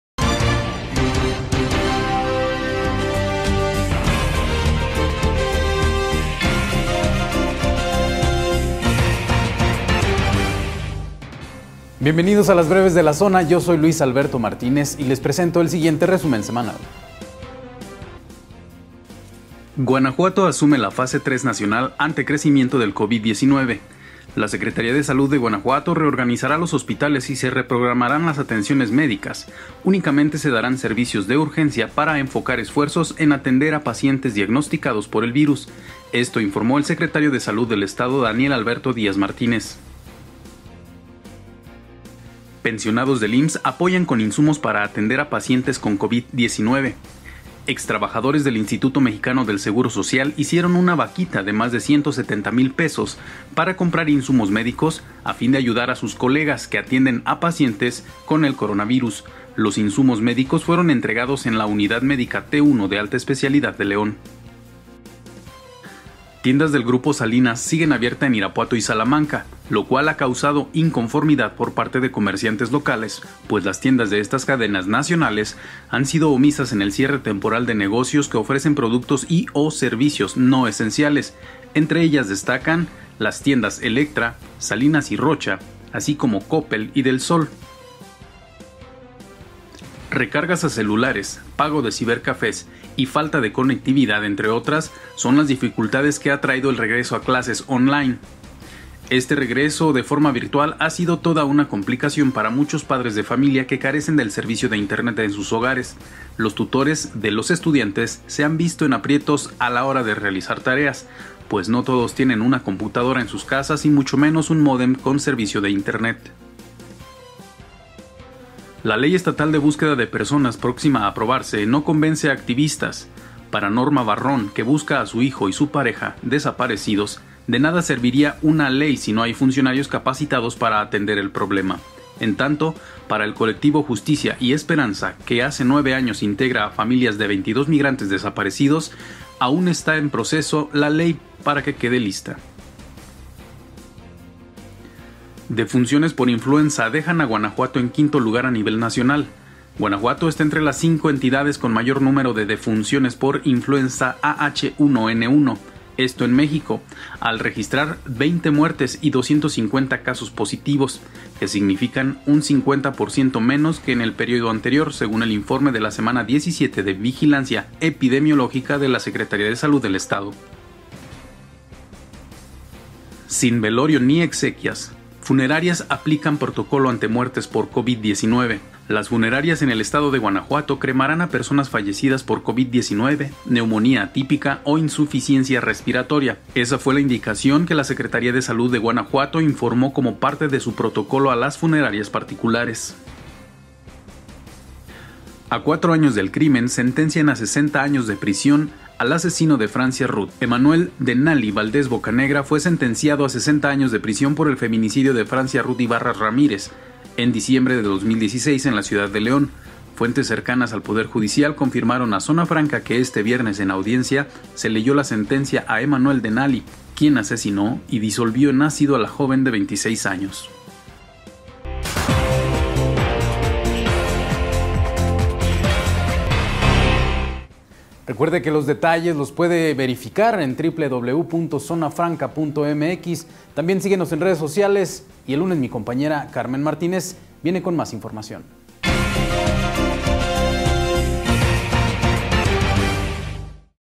Defunciones por Influenza dejan a Guanajuato en 5° lugar nacional | Breves de la Semana
BrevesDeLaZona📰 | No te pierdas el resumen de la semana
- Pensionados del IMSS apoyan con insumos para atender a pacientes con COVID-19.
- Tiendas del Grupo Salinas Pliego siguen abiertas en Irapuato y Salamanca.
- Recargas a celulares, pago de cibercafés y otras dificultades de las clases ‘online’.
- La Ley estatal de Búsqueda de Personas, proxima a aprobarse, no convence a activistas.
- Defunciones por Influenza dejan a Guanajuato en 5to lugar a nivel nacional.
- A cuatro años del crimen, sentencian a 60 años a asesino de Francia Ruth
- Sin velorio, ni exequias: funerarias aplican protocolo ante muertes por COVID-19.
- Guanajuato asume la Fase3 nacional ante crecimiento del COVID19.

No te pierdas nuestro resumen⤵️